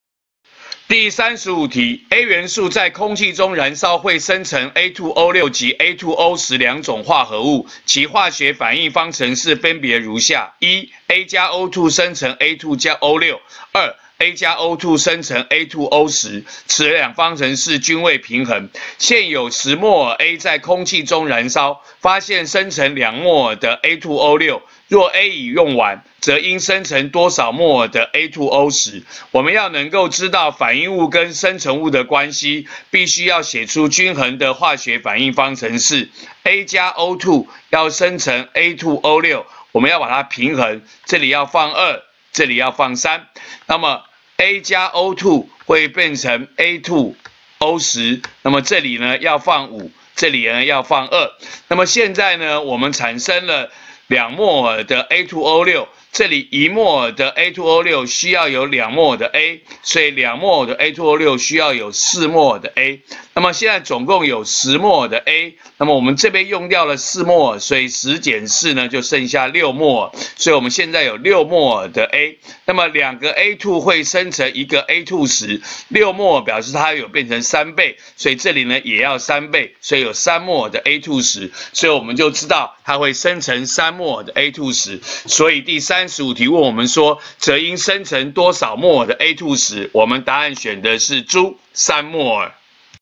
105學年下8年級理化一段第35題
A元素可生成兩種氧化物，給未平衡方程式。今有10莫耳A，氧化生成2莫耳某氧化物，問可生成另一氧化物多少莫耳？